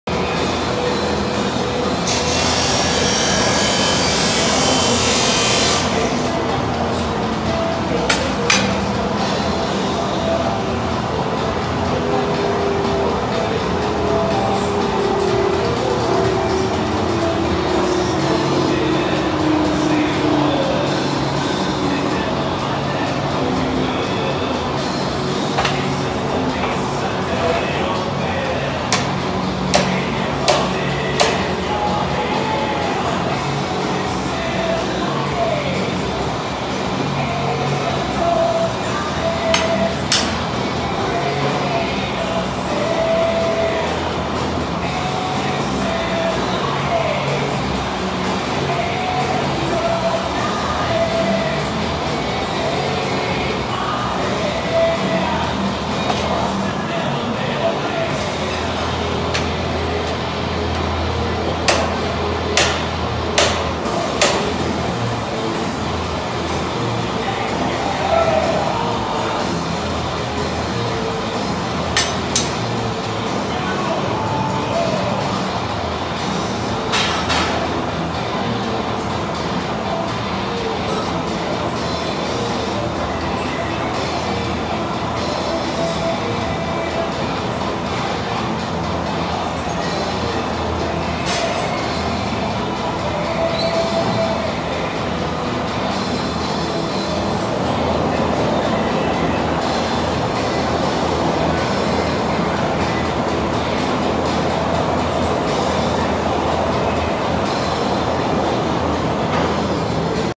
Making giant torsion spring the old fashion way
Oil quenching giant torsion spring and setting myself on fire. 🤦🏻‍♂️